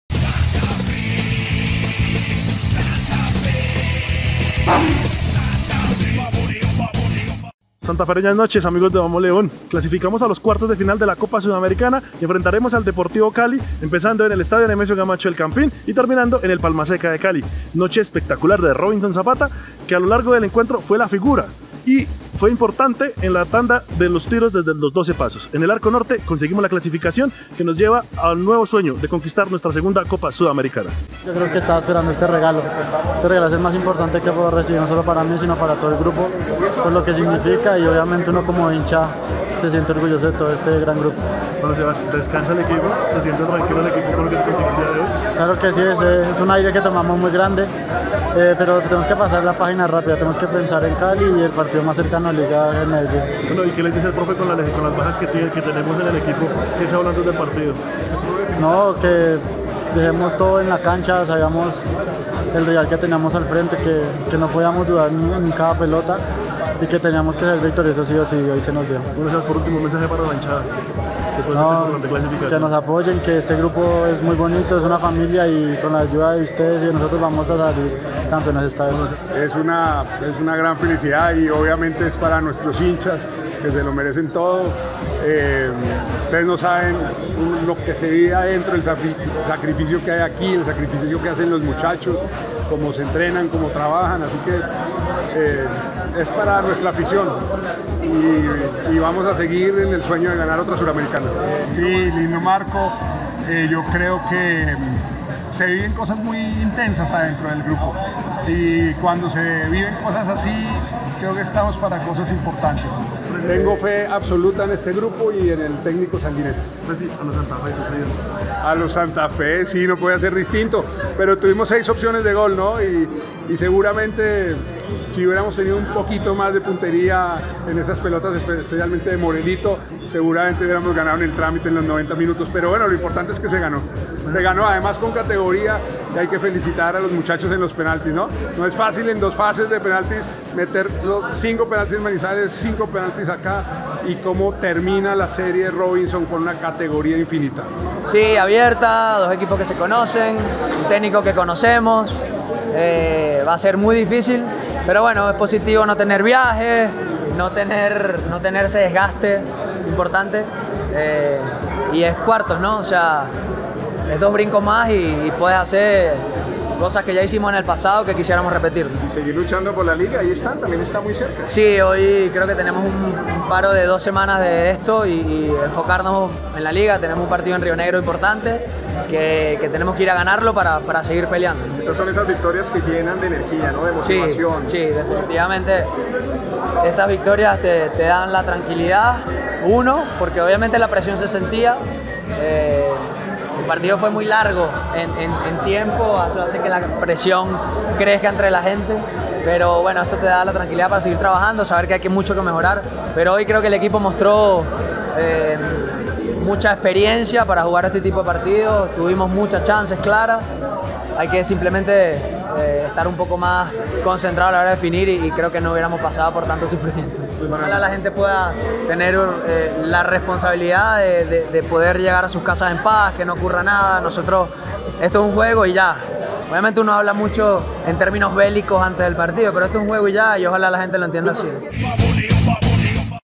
Declaraciones de la Clasificación a la siguiente Fase de Copa Sudamericana
El Presidente Juan Andrés Carreño, Luis Manuel Seijas y Sebastian Salazar los tres hinchas del rojo nos dan sus declaraciones frente a la Victoria y paso a la siguiente Fase de Copa Sudamericana.